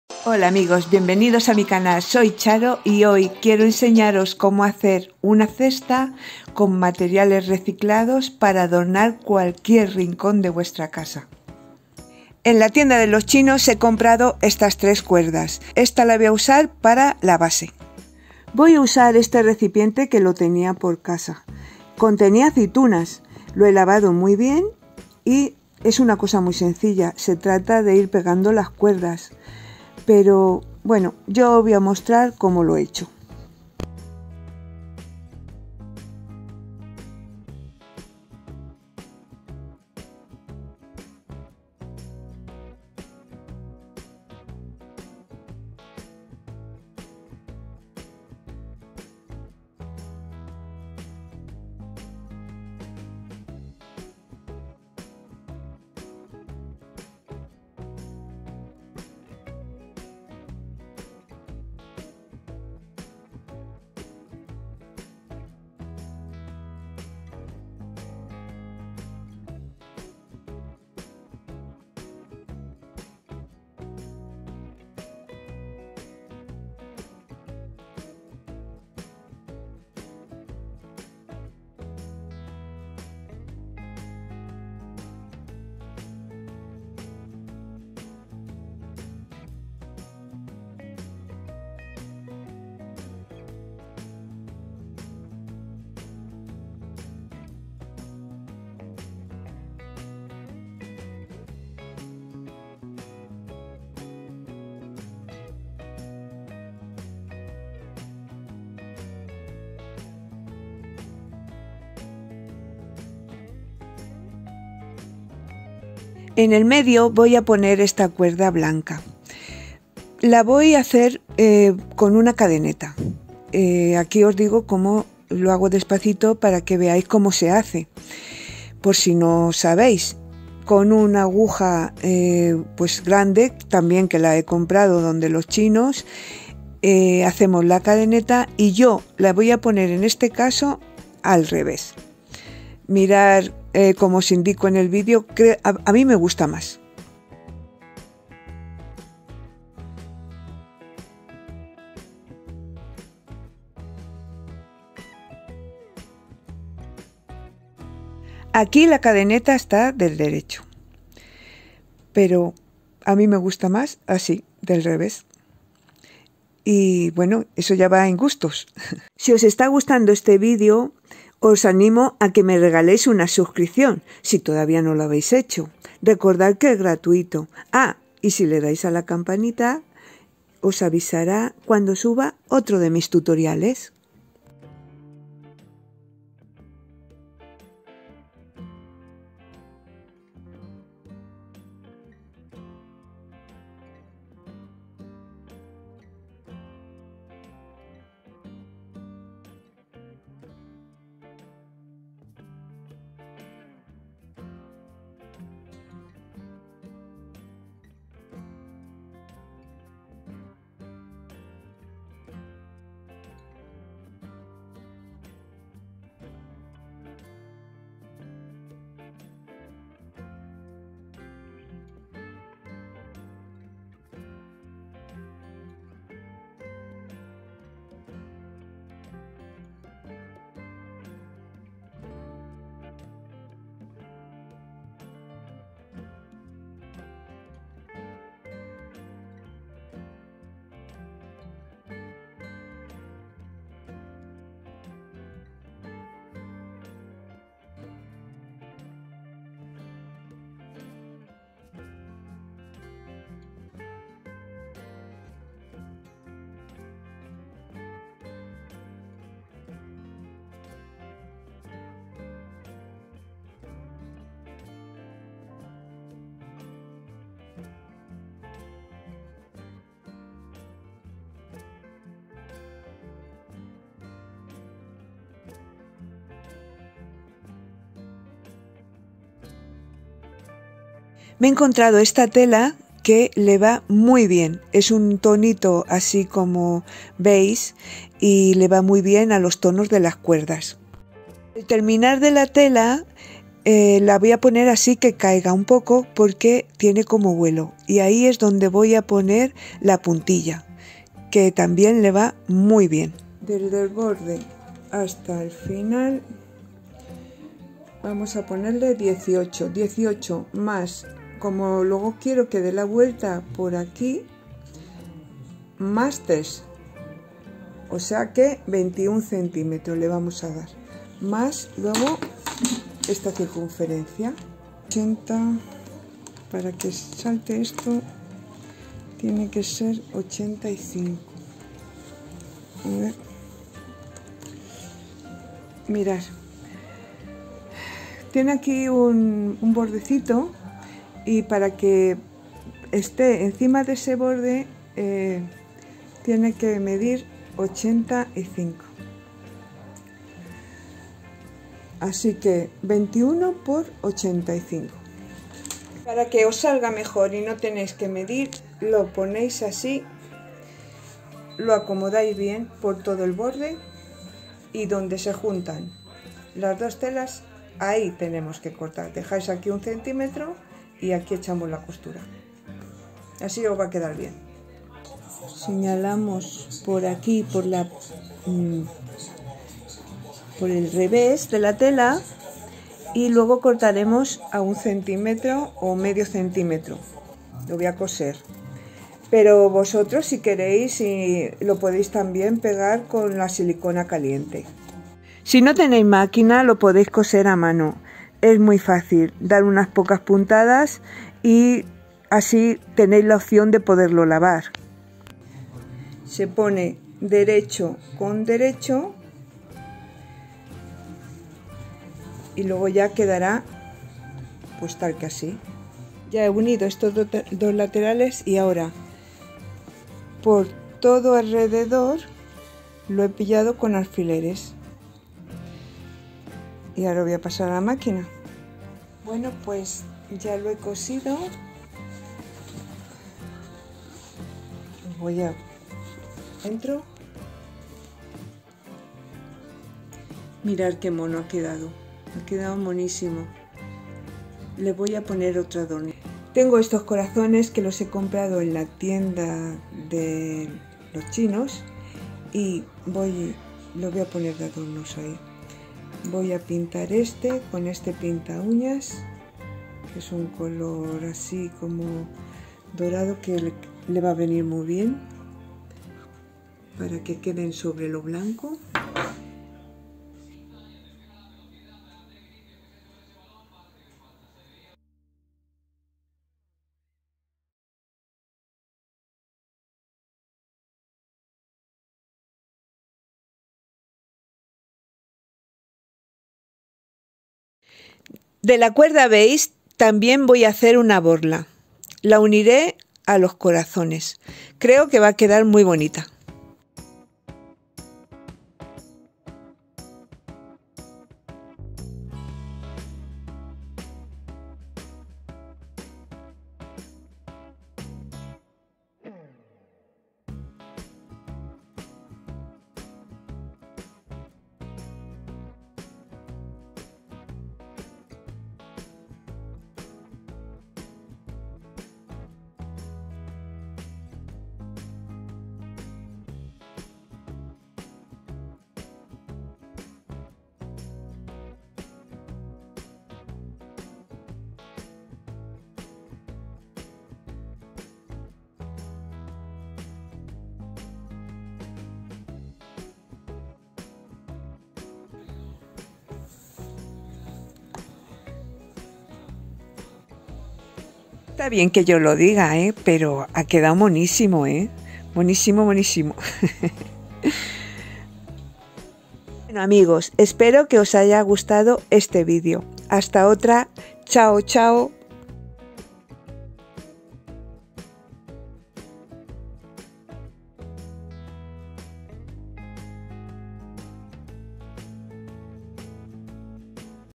CESTA CON CUERDAS, muy !! BONITA !! animate a hacerla le daras muchisima utilidad....:)
Con pocos materiales podras hacer esta !! PRECIOSIDAD !!, un recipiente que lo vamos a reciclar y con unas cuerdas y poco mas .

Con este gesto me ayudaras a subir mas tutoriales...hasta el proximo. chao chao ,,,:)